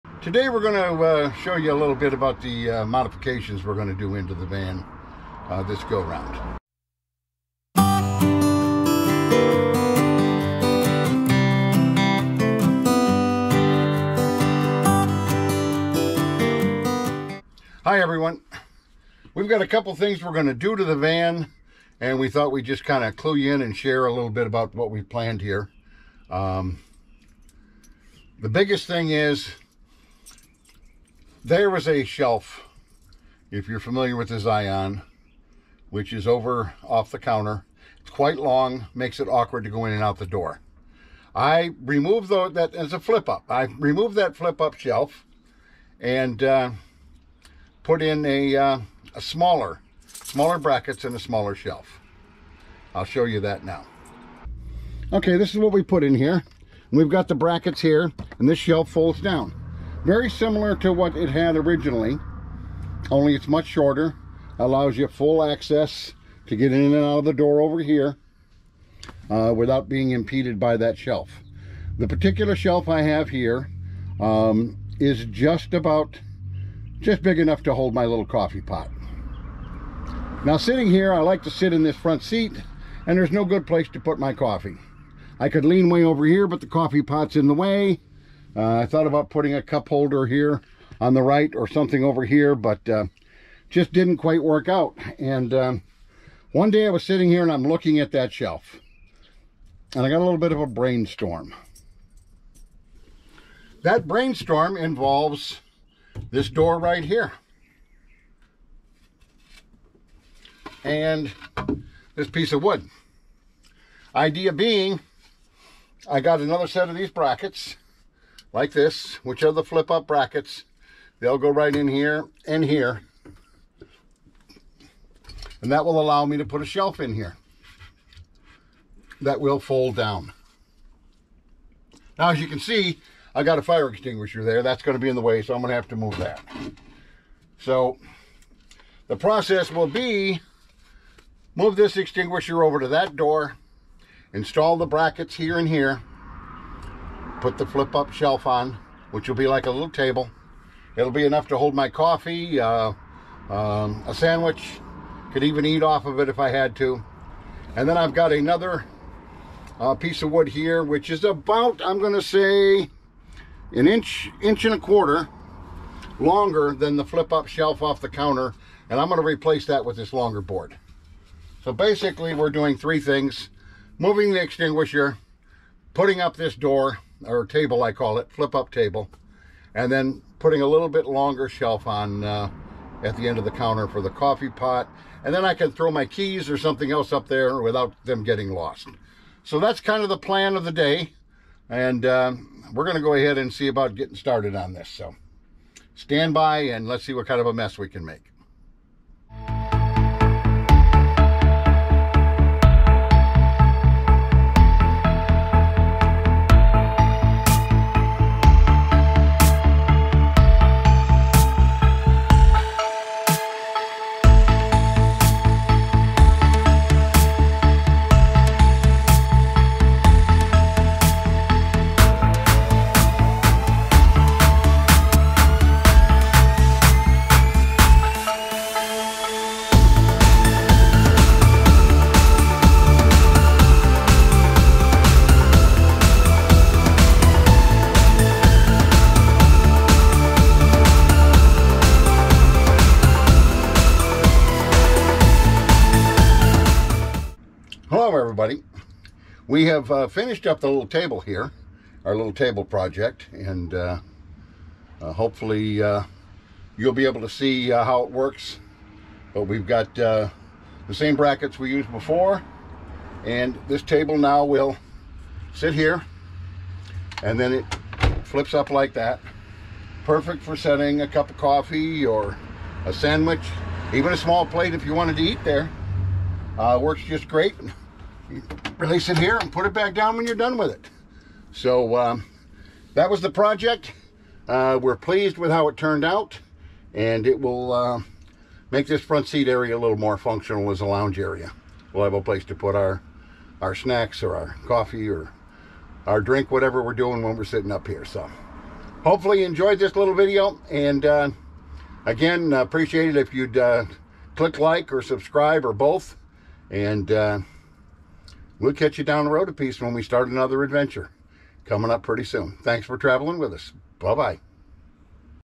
Flip Up Table in front Lounge Zion SRT
I call the front passenger seat in my Zion SRT by Roadtrek my Lounge Chair.   I enjoy rotating the passenger seat and relaxing with a coffee, book or my phone.  Only issue, no place to put those things!!   One day, saw my folding shelf and the light bulb lit up in my head!   This is the result!